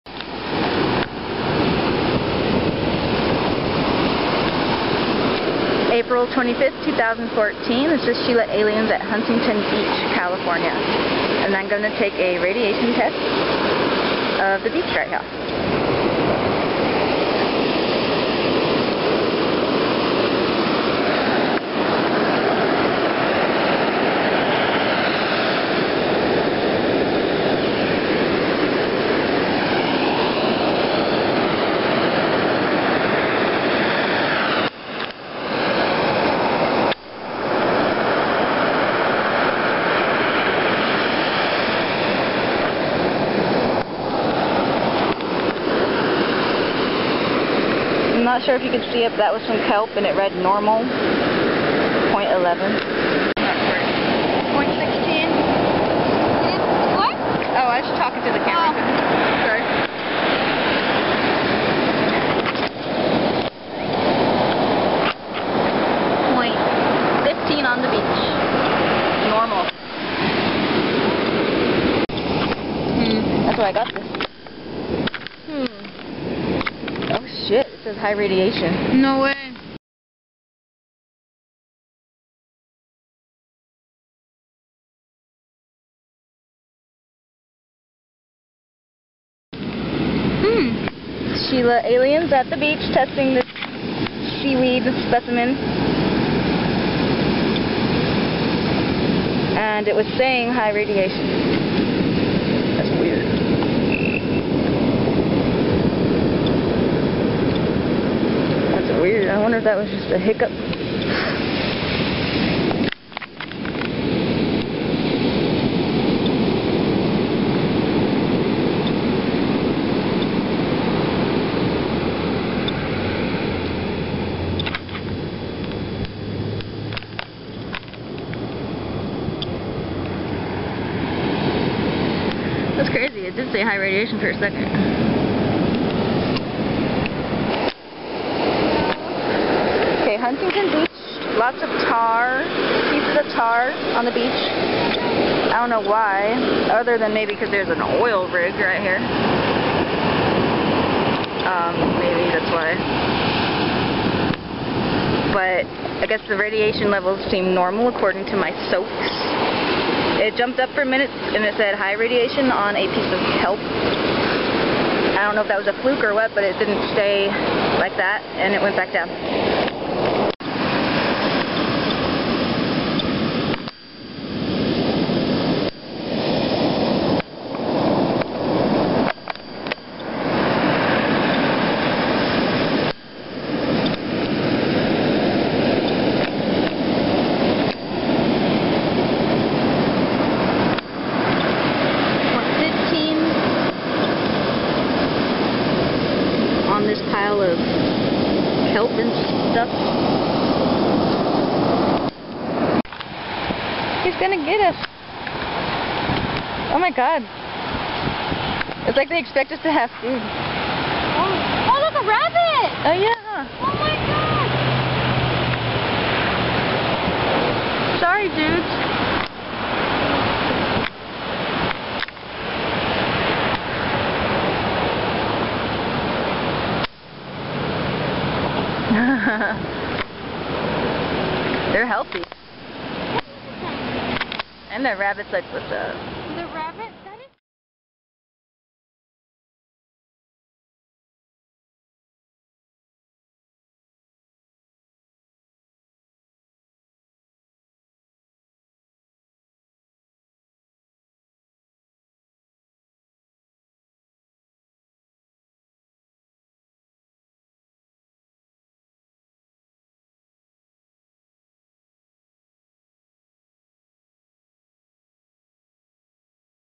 Huntington Beach, CA Radiation Test (+ Tar Patties) - 4/24/14
Geiger counter being used is a Soeks 01M. Overall normal readings aside from the one "possible fluke" high reading. I realize that next time I need to leave the geiger counter still the entire time until it has completed its reading, I'm learning as I go so cut me a break :P

I'll continue testing at various beaches in the L.A. area.  This was made possible by beautiful people such as yourself!

Please help fund my on-going efforts to test for radiation here in SoCal:

miaow :3